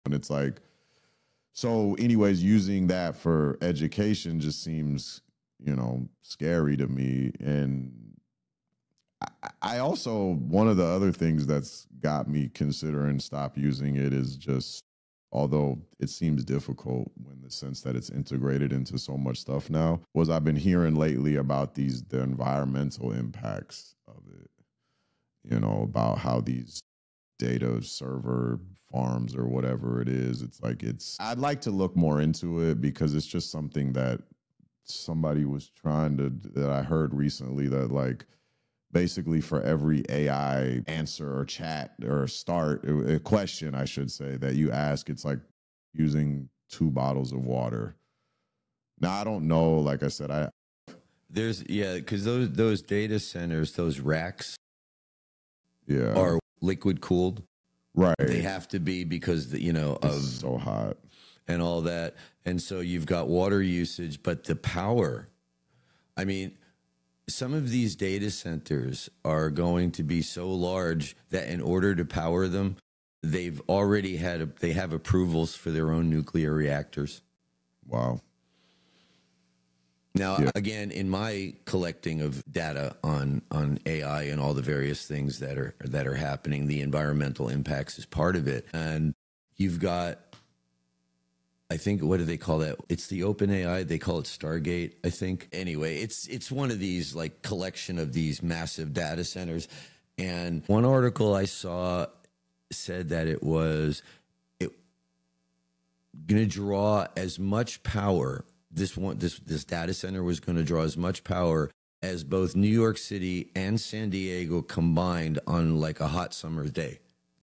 AI’s Environmental Impact: Data Centers & Water Usage
Every AI query costs the planet something — and we rarely count the gallons behind our “smart” questions.